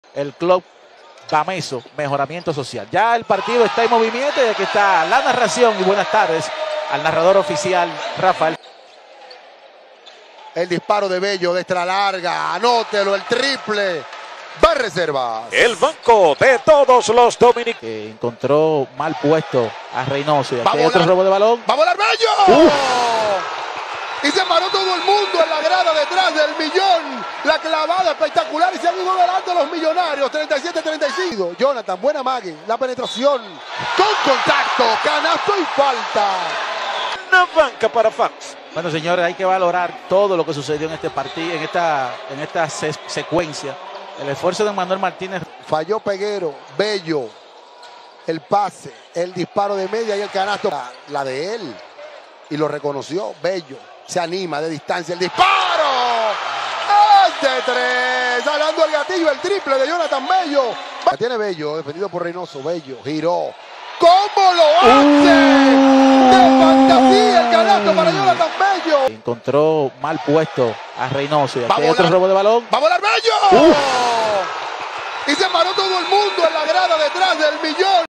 JONATHAN BELLO prendiendo el TBS Distrito 2024
El refuerzo del Club el Millón Jonathan Bello encabezó la primera victoria de su equipo y en sus dos partidos promedia 22 puntos.